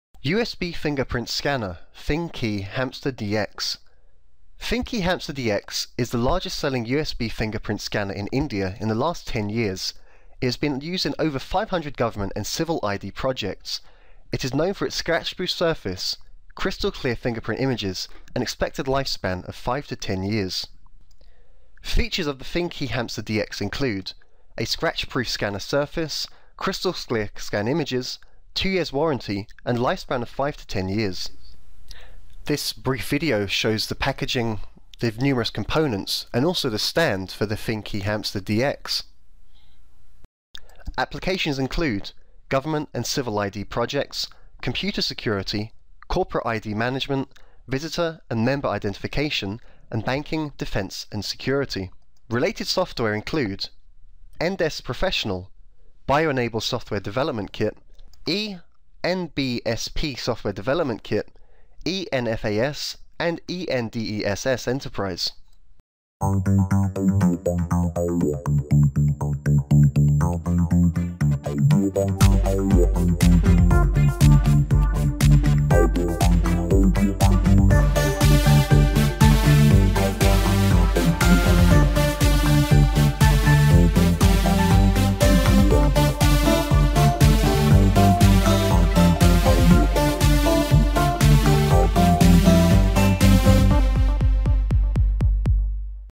USB Fingerprint Scanners / Readers - Fingkey hamster DX for Government & Civil ID projects
USB Fingerprint Scanners / Readers - Fingkey hamster DX for Government & Civil ID projects.Fingkey Hamster DX is largest selling USB Fingerprint scanner in India  since last 10 years. It has been used in over 500 Government & Civil ID Projects. Its known for its scratch proof surface, crystal clear fingerprint images and expected lifespan of 5 to 10 years.